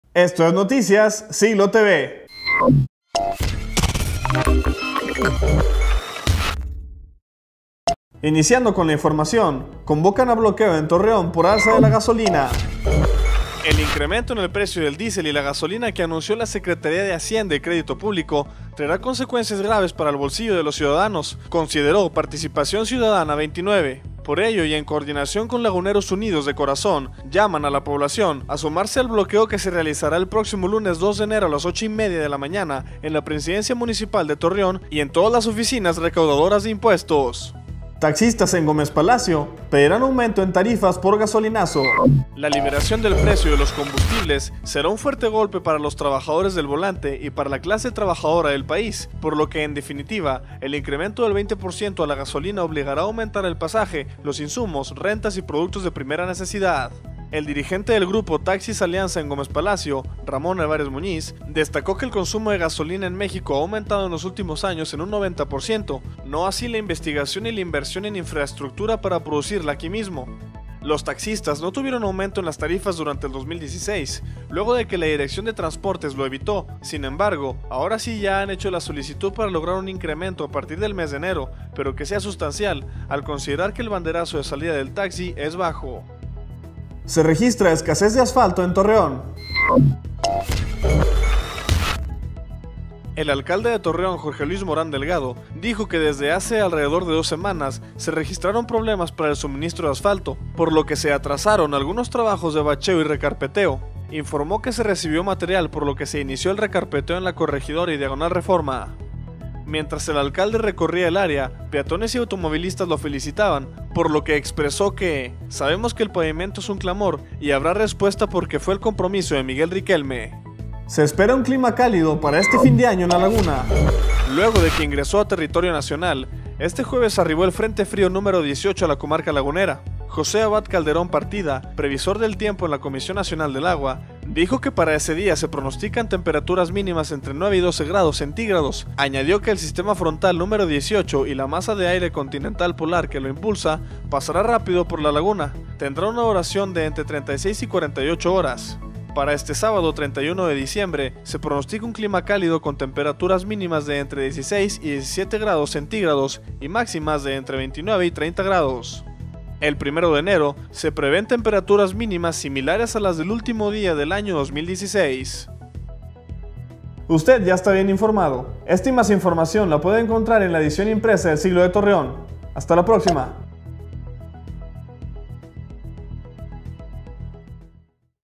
Convocan a bloqueos en Torreón por alza en gasolina
Pedirán aumento a tarifas por incremento de gasolina ::: Escasea el asfalto en Torreón ::: Se espera clima cálido para este fin de año en la Laguna